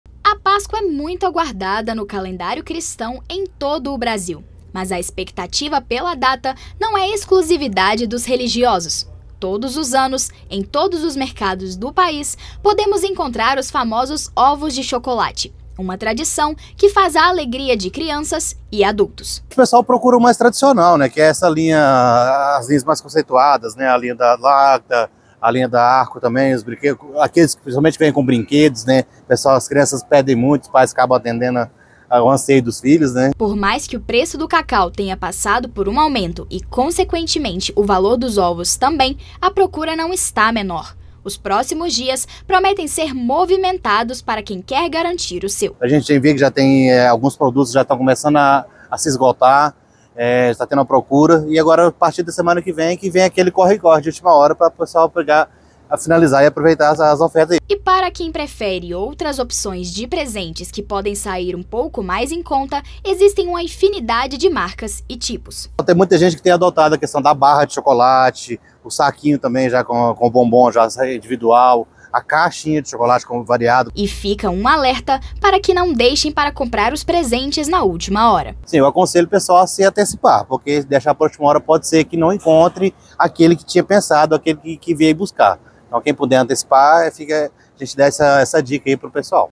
Preço do cacau faz com que ovos de Páscoa fiquem mais caros
Neste especial de Páscoa 2024, a NTV de Patos de Minas/MG mergulha na tradição dos ovos de chocolate, um símbolo que transcende gerações e religiões, trazendo alegria para crianças e adultos em todo o Brasil. Descubra as tendências deste ano, desde os clássicos ovos de Páscoa com brinquedos até alternativas mais em conta como barras de chocolate e caixas de bombons variados.
Com o aumento no preço do cacau impactando o valor dos ovos, a procura permanece alta, e os produtos começam a se esgotar. Preparamos dicas valiosas para evitar a correria de última hora e garantir que você aproveite as melhores ofertas. Não deixe para a última hora; antecipe suas compras e faça desta Páscoa um momento inesquecível.

A celebração da Páscoa vai além das fronteiras religiosas, transformando-se em um evento cultural amplamente esperado em todo o Brasil. Este período não apenas reúne famílias mas também movimenta o mercado com a tradicional venda de ovos de chocolate.
A Páscoa de 2024, conforme destacado pela NTV de Patos de Minas/MG, promete ser ainda mais especial, apesar dos desafios econômicos que influenciam os preços dos ingredientes, como o cacau. A procura por ovos de chocolate continua a crescer, evidenciando a força dessa tradição.

Os consumidores, buscando alternativas para presentes de Páscoa, voltam-se tanto para os clássicos ovos com brinquedos quanto para opções mais acessíveis, como barras de chocolate e caixas de bombons variados. A antecipação das compras surge como uma estratégia inteligente para evitar a correria e a escassez de produtos de última hora.

É essencial que os consumidores estejam atentos às ofertas e novidades, garantindo assim que consigam adquirir seus presentes preferidos sem imprevistos.

Observa-se que a expectativa pela Páscoa transcende as barreiras religiosas, unindo pessoas de diversas faixas etárias em torno da tradição dos ovos de chocolate. Este símbolo de alegria e celebração tem sua demanda mantida alta, mesmo diante dos desafios econômicos, como o aumento no preço do cacau.

A preferência do público por ovos de Páscoa tradicionais, especialmente aqueles que acompanham brinquedos, reflete não apenas um desejo por chocolate mas também por uma experiência afetiva e memorável.
A emergência de alternativas mais acessíveis, como barras de chocolate, bombons individuais e caixas de chocolates variados, indica uma adaptação do mercado e dos consumidores às novas realidades econômicas.

A recomendação para que as compras sejam antecipadas, evitando a correria e a possível falta de produtos desejados, reitera a necessidade de planejamento e organização.

Por que a Páscoa é tão aguardada no Brasil?
A Páscoa é aguardada não apenas por seu significado religioso, mas também pela tradição dos ovos de chocolate, que traz alegria para crianças e adultos em todo o país.

Quais são os ovos de Páscoa mais procurados?
Os ovos de Páscoa mais procurados são os tradicionais, especialmente aqueles que vêm com brinquedos dentro.

Como o preço do cacau influencia a compra de ovos de Páscoa?
Apesar do aumento no preço do cacau e, consequentemente, dos ovos de chocolate, a procura por esses itens não diminuiu.

Como está a disponibilidade dos produtos de Páscoa?
Alguns produtos de Páscoa já estão começando a se esgotar, indicando uma alta procura, especialmente nas semanas finais antes da celebração.

Existem alternativas mais em conta aos ovos de Páscoa?
Sim, há diversas alternativas mais acessíveis, como barras de chocolate, saquinhos com bombons individuais e caixas de chocolate com sabores variados.

Qual é a recomendação para a compra de presentes de Páscoa?
A recomendação é que as compras sejam antecipadas para evitar a correria de última hora e a possibilidade de não encontrar o produto desejado.